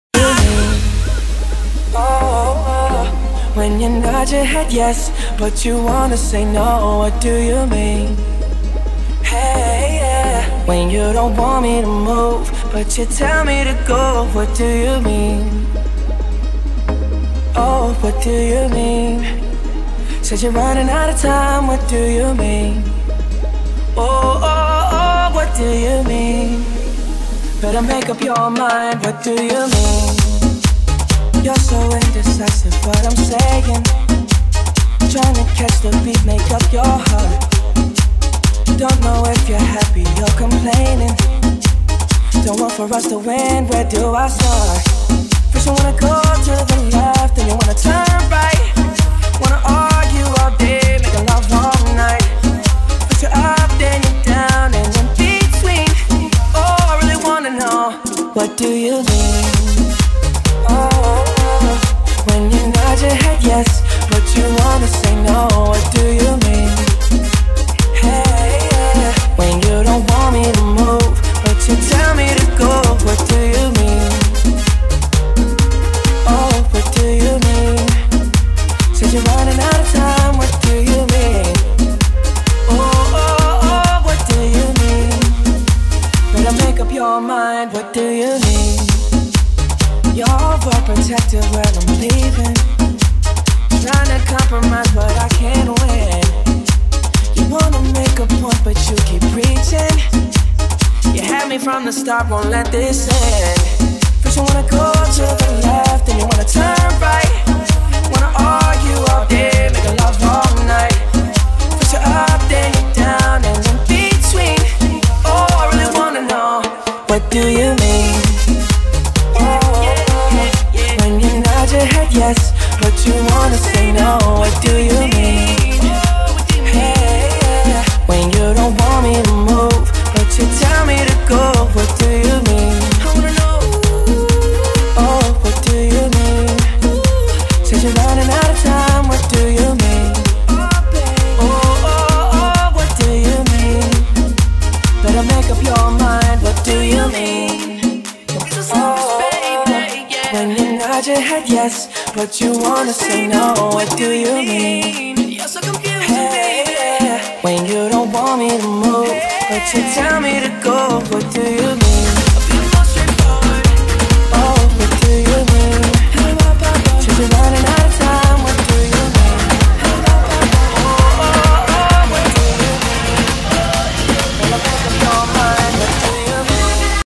16. Sesion Noviembre 2015 Dj Rajobos
Ya Disponible Sesion Noviembre 2015 de Dj Rajobos Cargada con 22 Auntenticos Temazos del Mejor Sonido Reggaeton - Latin House - Comercial & EDM para que lo Disfrutes a Tope ^^